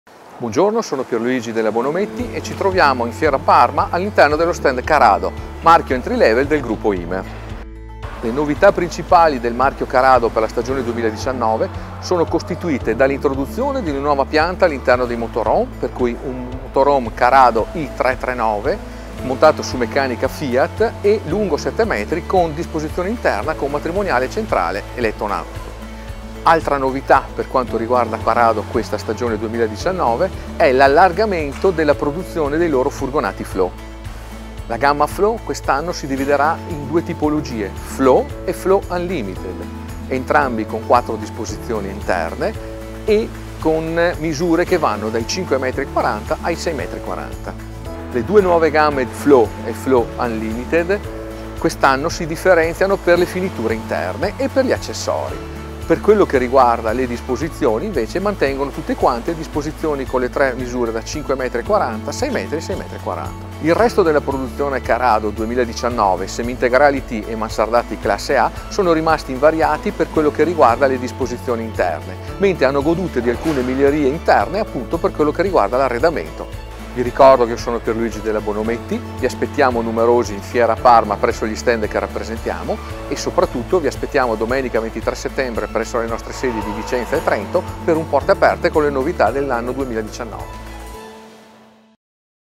Salone del Camper 2018: Novità CARADO presentate da Automarket Bonometti
Pierluigi ci porta alla scoperta delle novità di Carado per la collezione 2019 con un video girato direttamente al Salone del Camper di Parma 2018.